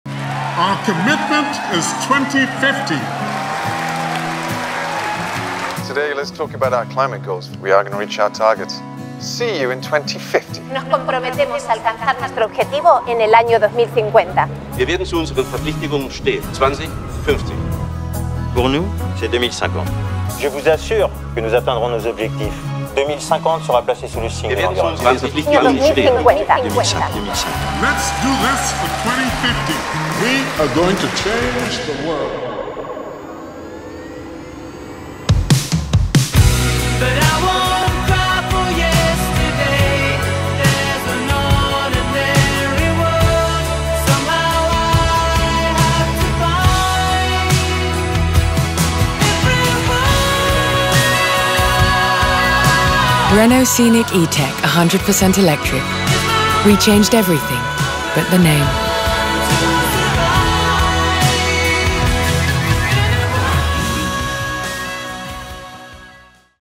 Renault Scenic E-Tech Electric
Der neue Renault Scenic ETech Electric, Auto des Jahres 2024, ist jetzt beim Händler verfügbar.

Komfort im Fahrzeug: Selbstdimmendes Panorama-Glasdach Solarbay®. Schalldämmung mit Kokon-Effekt. Hi-Fi-System mit 9 Harman Kardon® Lautsprechern. Auf der Rückbank: Intelligente hintere Armlehne Ingenius® für Konnektivität mit Tablet oder Smartphone. Durchgehend ebener Boden mit 27,8 cm Kniefreiheit. 38 l Stauraum für persönliche Gegenstände. Kofferraumvolumen von 545 – bis 1670 l.

Wir haben alles geändert, außer den Namen!